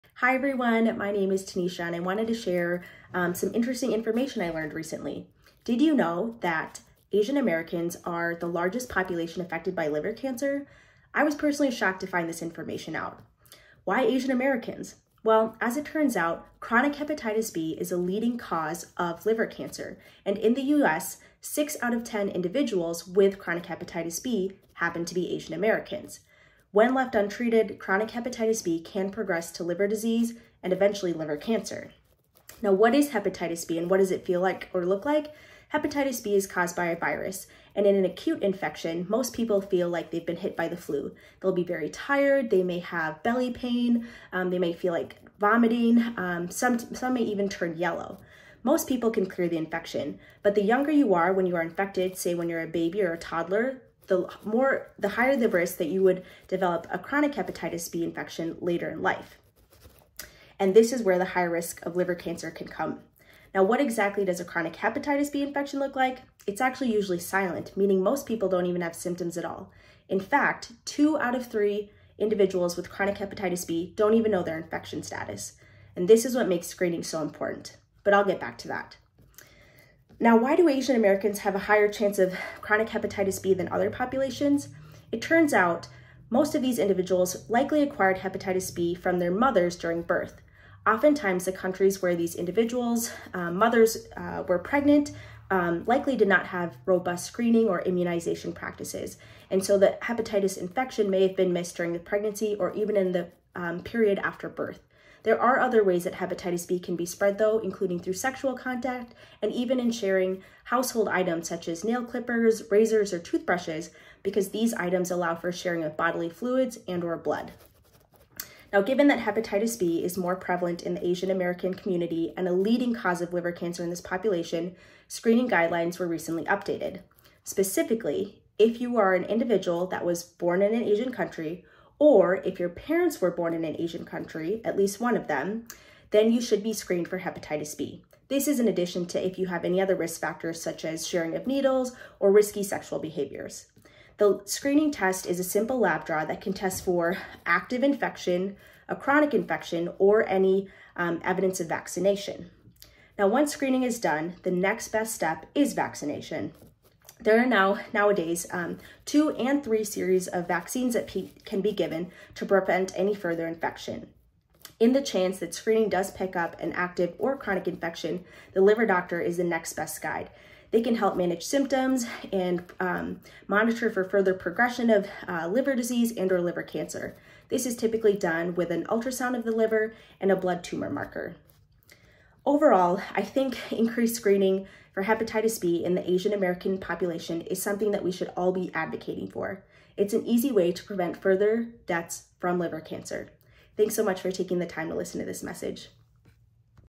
What's 'Hep'pening with Asian Americans and Liver Cancer?, Tanisha Ronnie, MD
For our 2023 Poster Competition, Tanisha Ronnie, MD of Loyola University presents "What's 'Hep'pening with Asian Americans and Liver Cancer?" Dr. Ronnie was mentored by Jonah Rubin, MD.